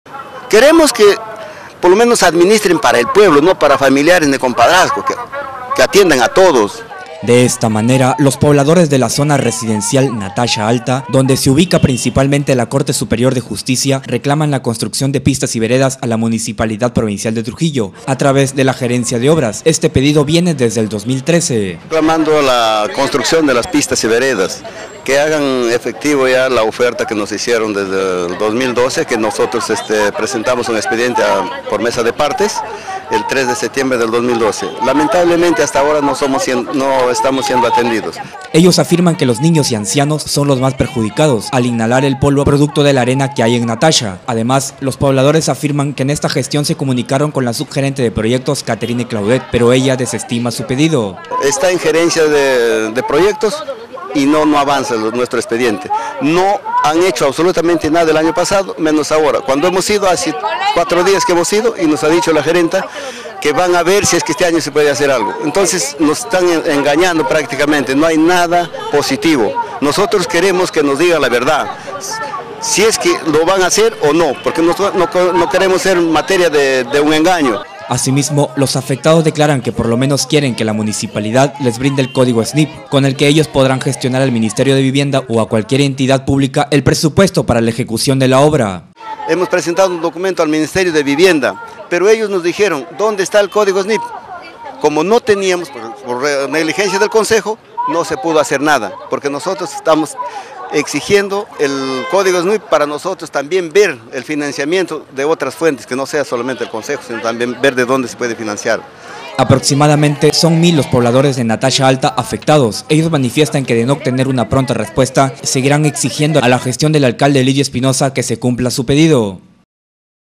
Natasha Alta exigen construcción de pistas y veredas - Trujillo
Pobladores de la zona residencial Natasha Alta donde se ubica principalmente la corte superior de justicia reclaman la construcción de pistas y veredas a la MPT a través de la gerencia de obras.